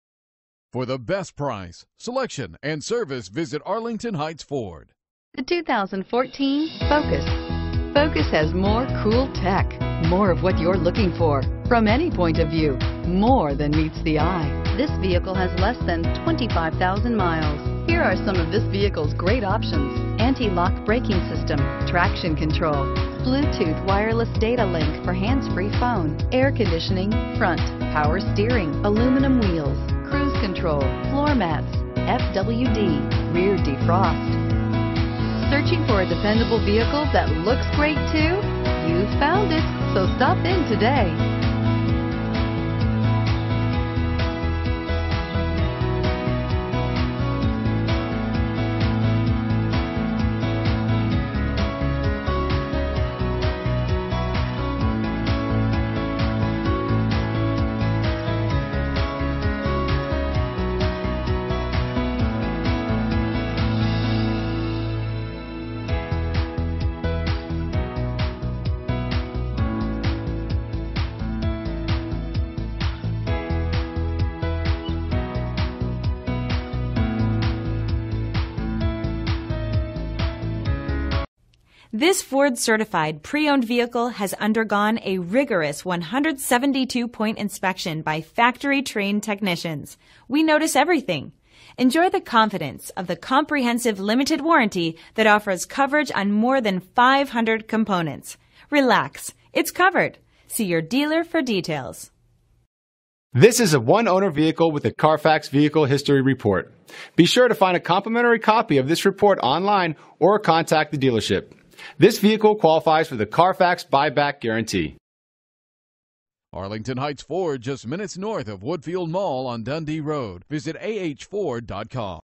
2014 Ford Focus Live Arlington Heights IL A162399A
2014 Ford Focus SE

801 Dundee Rd.

2.0L 4-Cylinder, Auto, FWD, Alloy Wheels, Cruise Control